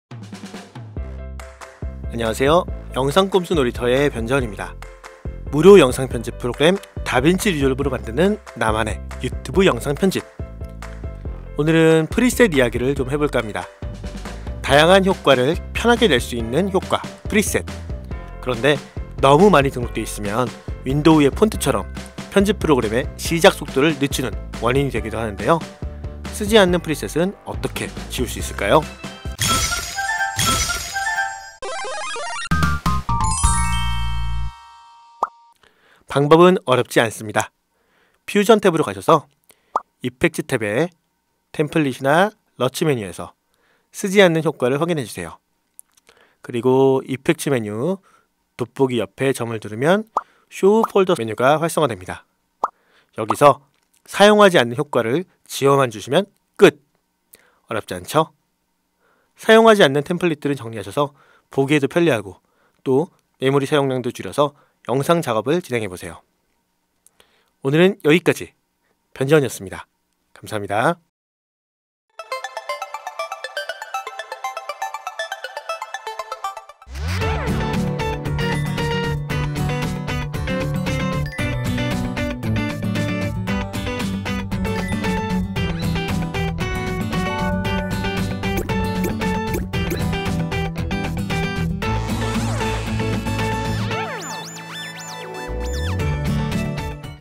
30초컷! 안쓰는 템플릿! 지워버리세요! - 무료 영상편집프로그램 다빈치리졸브
안녕하세요! 다빈치리졸브 로 시작하는 유튜브 동영상편집 강의 !
변자운입니다 :)

오늘은 템플릿 관련된 이야기를 좀 할까 합니다.
템플릿은 미리 다양한 효과 나 자막 을 만들어둔 다음,
사진이나 내용만 바꿔서 화려한 효과를 만들어주는 일종의 사전제작물인데요,

물론 편리하긴 하지만, 막 깔다보면 너무 효과가 많아져서
쓰기도 힘들고, 또 다빈치리졸브의 시작속도도 느리게 만드는 경우가 있습니다.

이럴땐.. 어떻게 하면 좋을까요?
오늘은 30초만에 배워보는! 이런 번잡한 템플릿들을 깔끔하게 정리하는
법에 대해 알아보겠습니다 :)

이태원참사 희생자 분들의 명복을 빕니다